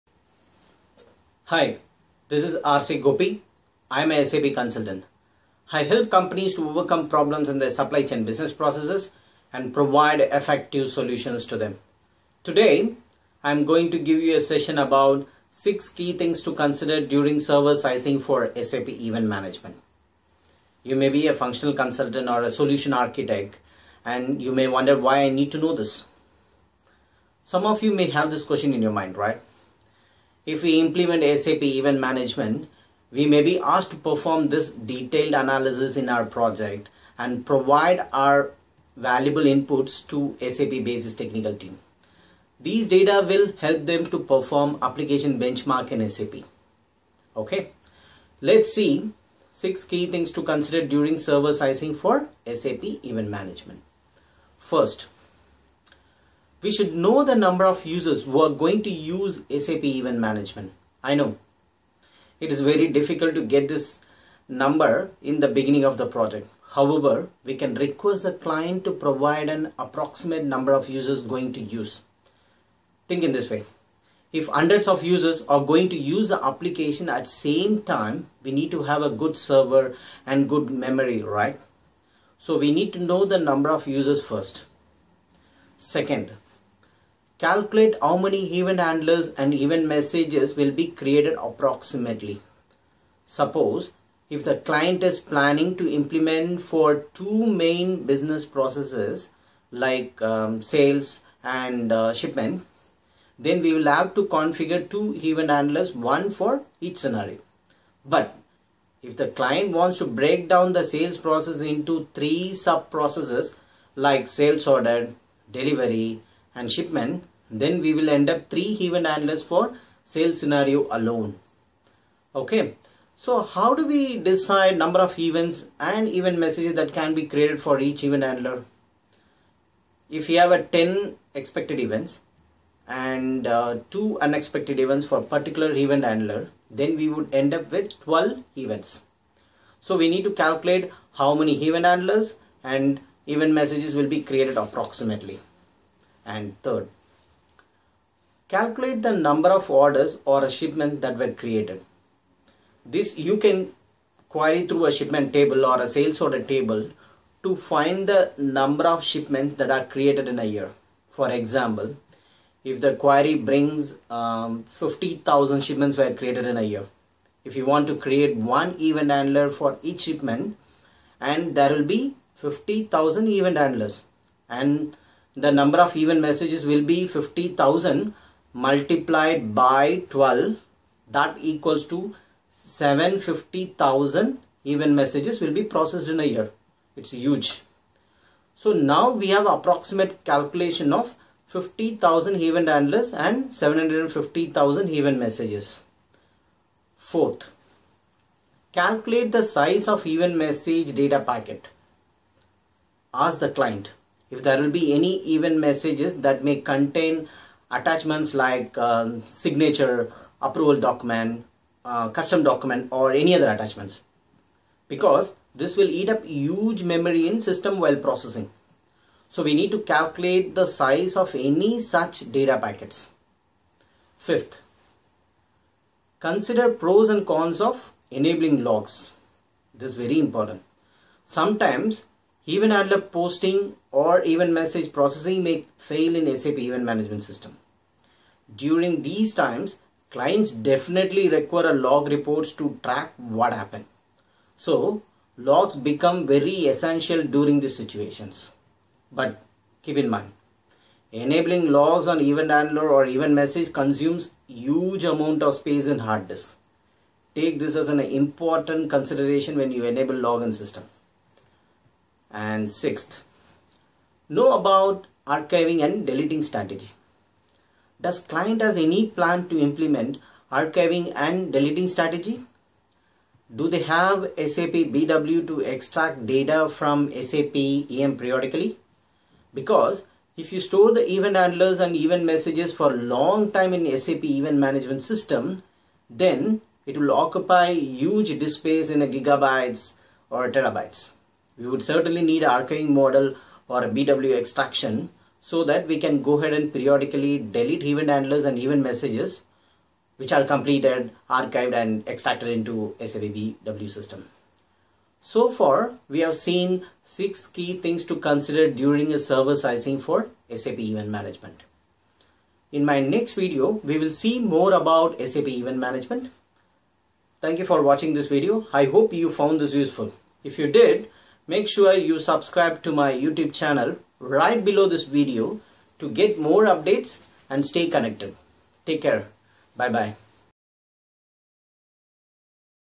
SAP Event Management - Six Key Things To Consider During Server Sizing
SAP Event Management - Six Key Things To Consider During Server Sizing: This video helps Functional Consultant / Solution Architect to perform detail analysis and provide inputs to SAP Basis technical team to plan for SAP Event Management server sizing and to perform Application Benchmark in SAP.